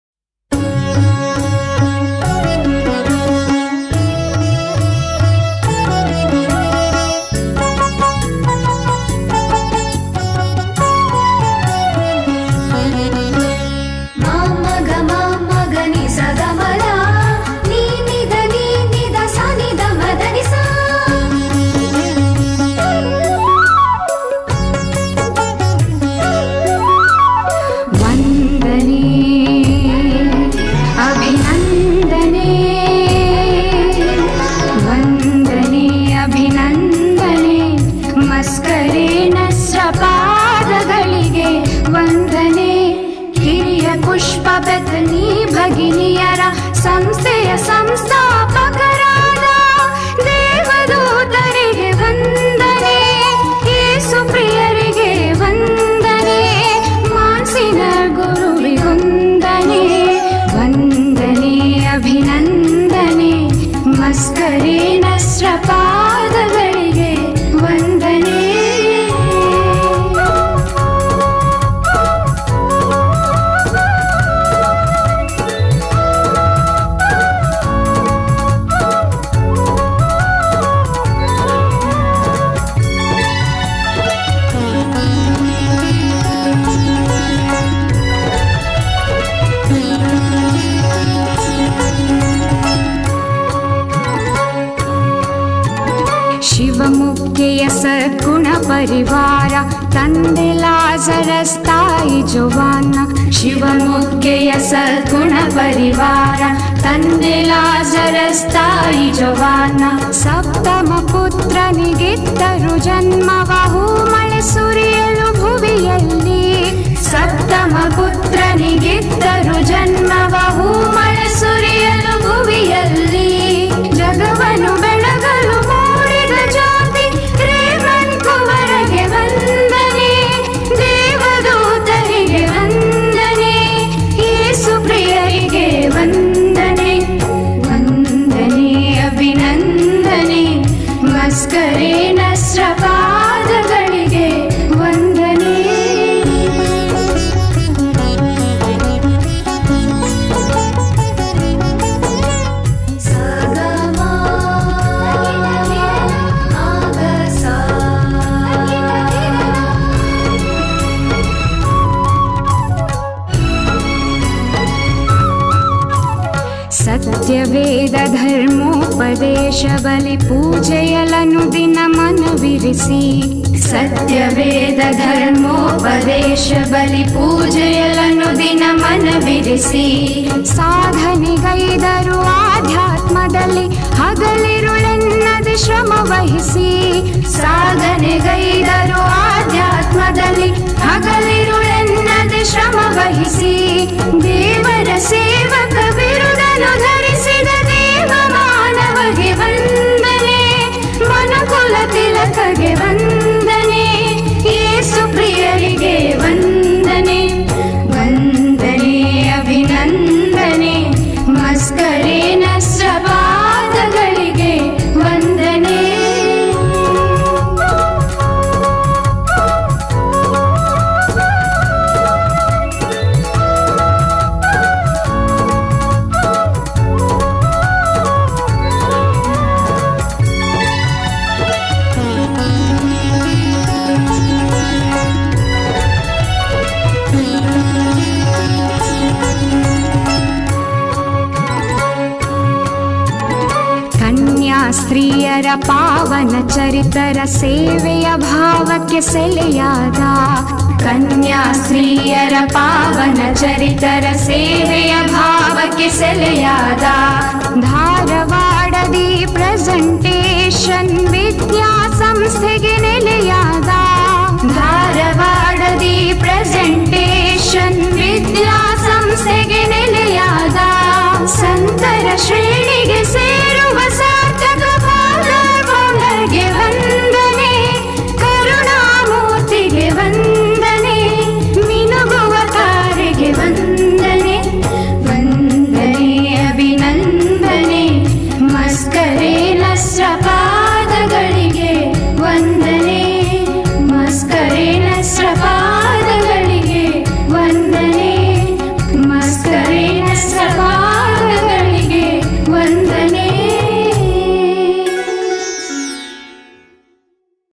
Champion in Education - RFC Mascarenhas
The memory of the Servant of God RFC Mascarenhas, the Founder of Bethany Congregation and the Bethany Educational Society is fresh in us after many years of his going to his eternal abode, it because of his zeal in educating the poor and marginalized and paying special attention to the girl children. We salute him for his tremendous work in the development of education.